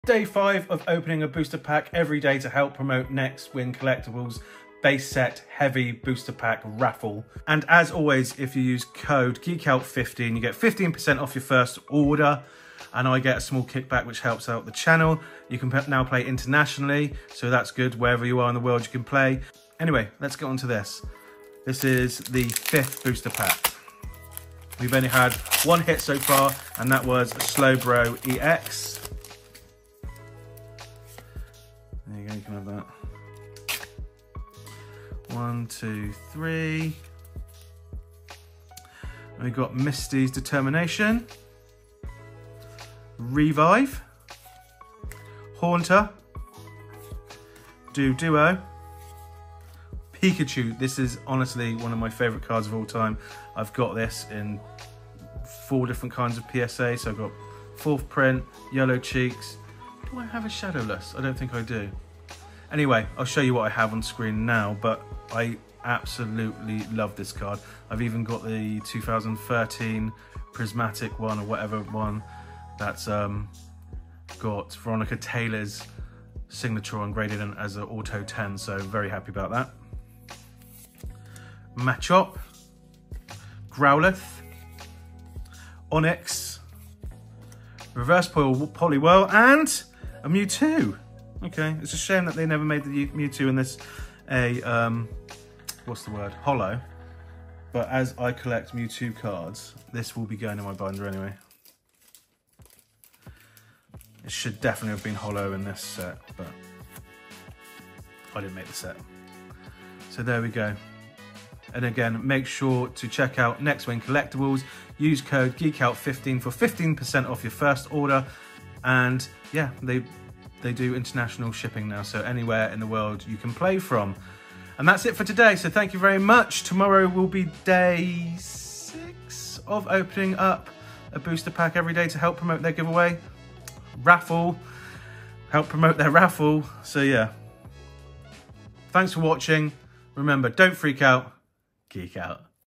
5 of 8 Evolution Booster RIP in colab with NXT WIN Collectables
Day 5 of opening a Pokemon evolutions booster pack everyday for 8 days to help promote NTX WIN collectables heavy base set booster pack raffle!

My Pokémon TCG live name is Rayquazaking200 so add me if you want!

My Pokemon go code is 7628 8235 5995

Send mail or stuff too:
Geekout Industries
TA24 9DS